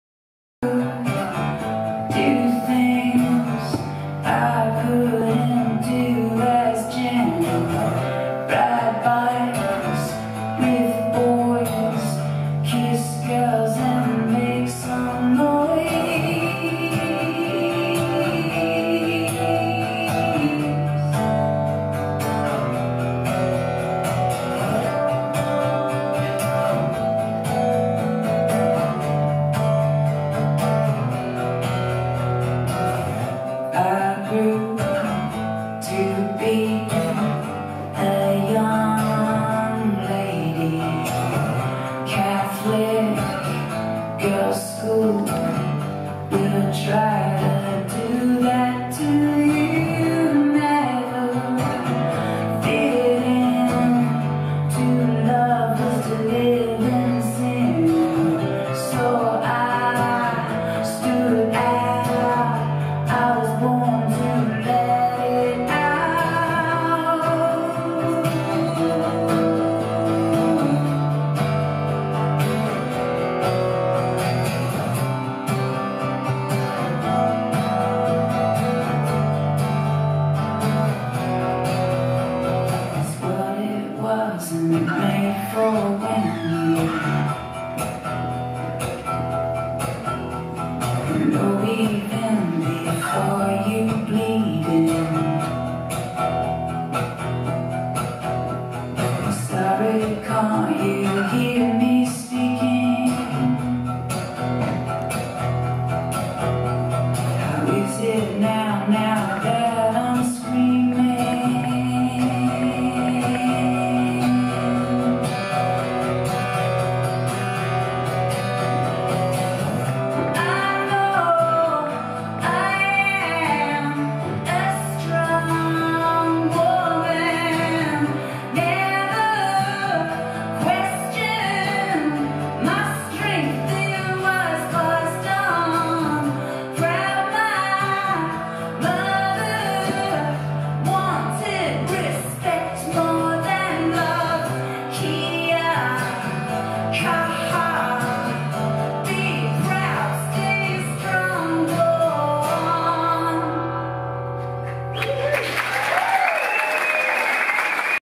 P1000187
Jen Cloher~ these lyrics are what I was like when I was young (except the kissing girls lyric)... unedited from tonight. Will post up more. A good sound, good camera, still learning. Love the music. Port town Hall  X